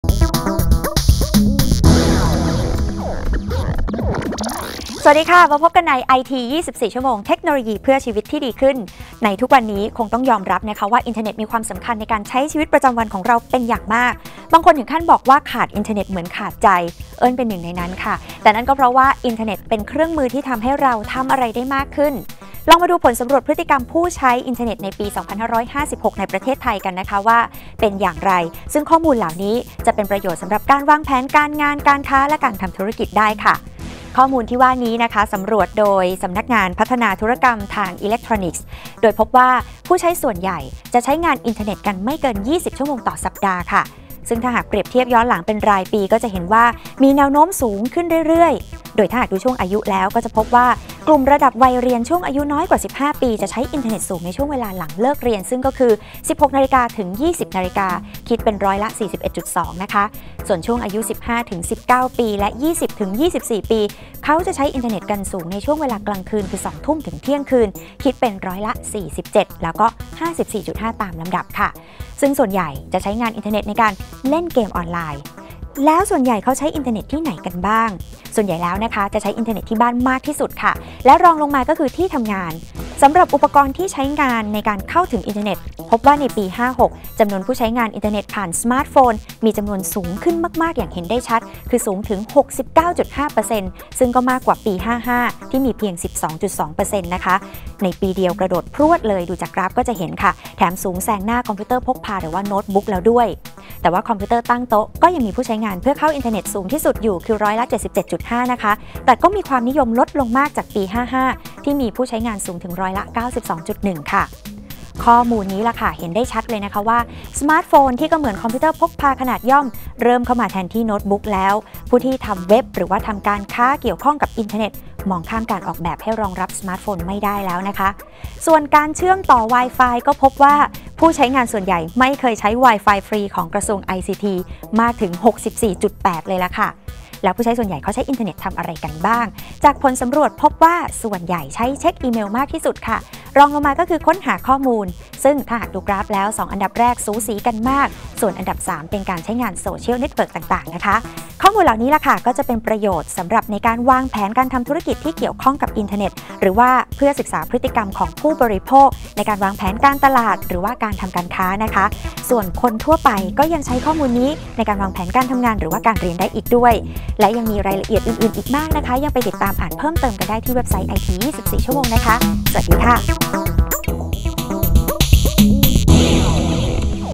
iT24Hrs ผลสำรวจพฤติกรรมผู้ใช้ internet ในไทย 2556 - 25july13
ข้อมูลเป็นประโยชน์สำหรับวางแผนการทำงาน การเรียน การทำธุรกิจ การตลาด
ผลสำรวจพฤติกรรมผู้ใช้ internet ในไทย 2556